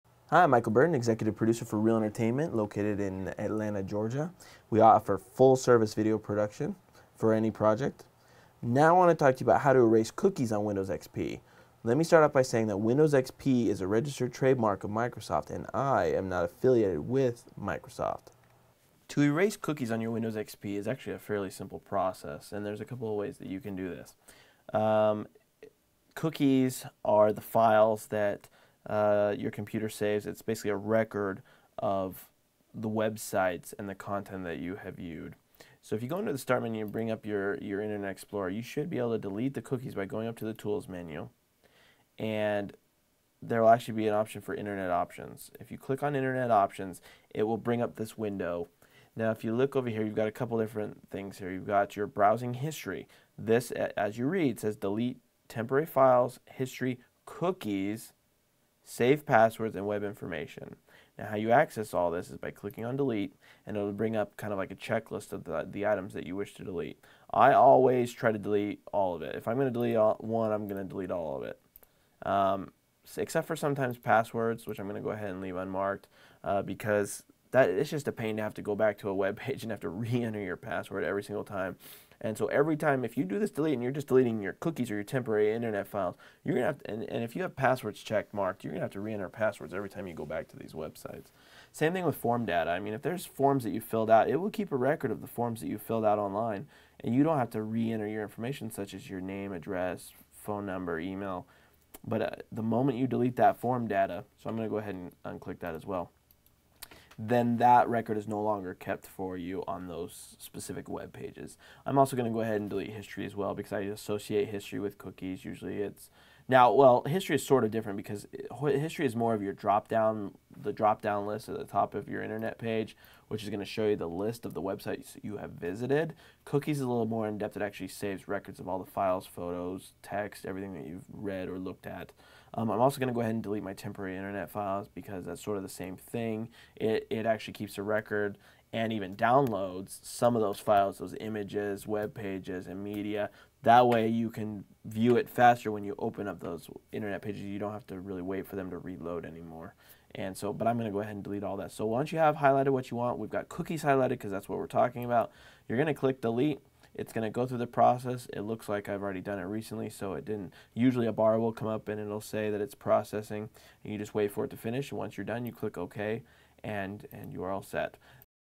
Windows XP : How to Erase Cookies on Windows XP
In order to erase cookies in Windows XP, click on the "Internet Options" section of a given Internet browser to find the desired option. Delete cookies and temporary Internet files with help from the executive producer of a video production company in this free video on erasing cookies in Windows XP.

Bio: Michael Burton has over 12 years of experience with PC computers.